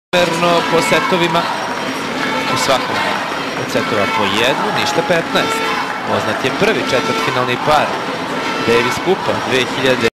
Дарко и Цаца гледају тенис (Србија - Швајцарска)
Дејвис куп меч, 1. фебруар 2014.

Darko i Caca gledaju tenis (Srbija - Švajcarska)